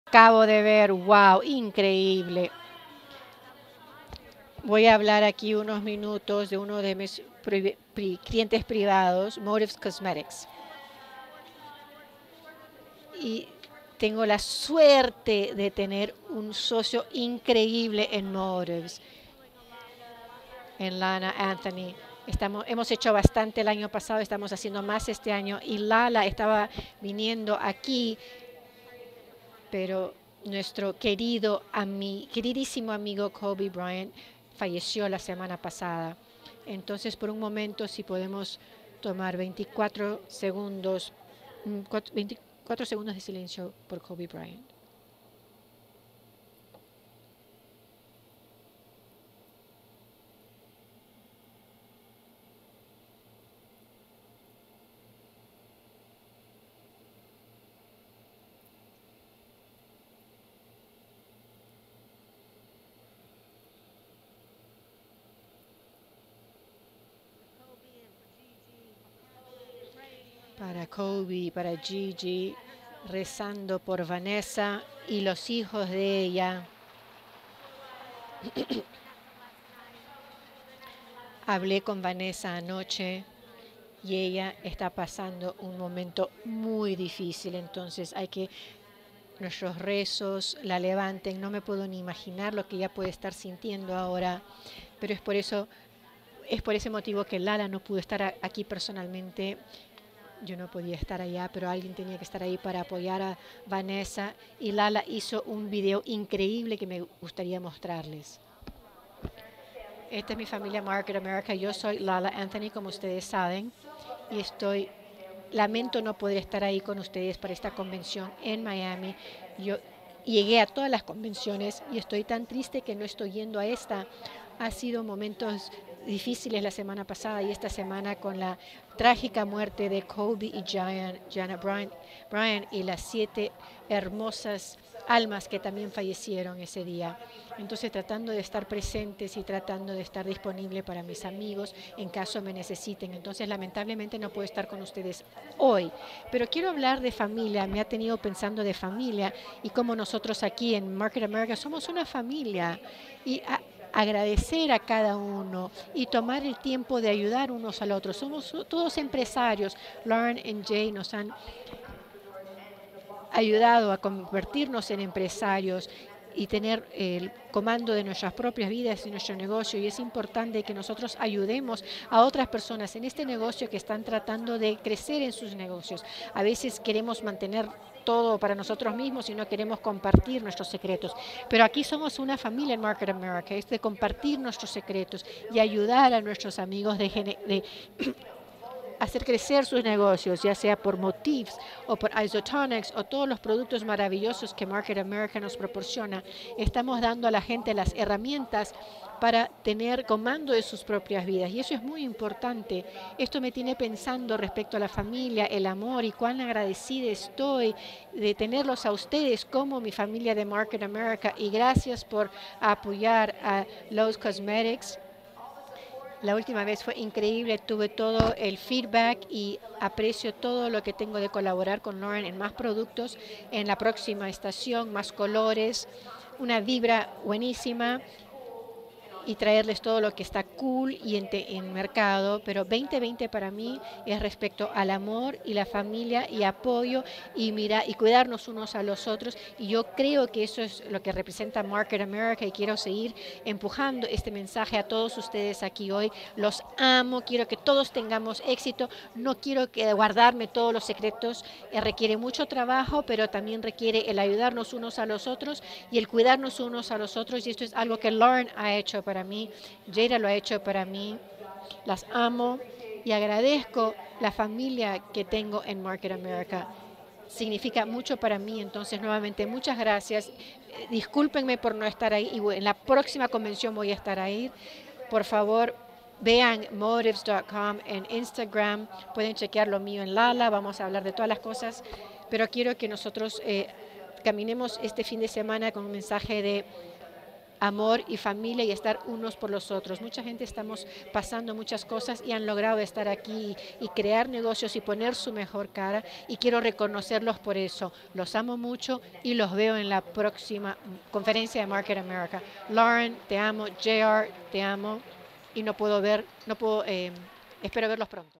Mensaje Especial | Loren Ridinger y La La Anthony
Loren Ridinger pidió un minuto de silencio en honor de Kobe y Gigi Bryant. La La Anthony envió un mensaje especial instándonos a darle un mayor sentido al amor, la familia y el trabajo en equipo tanto en los negocios como en la vida.

Loren Ridinger takes a moment of silence to honor Kobe and Gigi Bryant.  La La Anthony sends a special message urging for a greater sense of love, family and teamwork in both the business and life.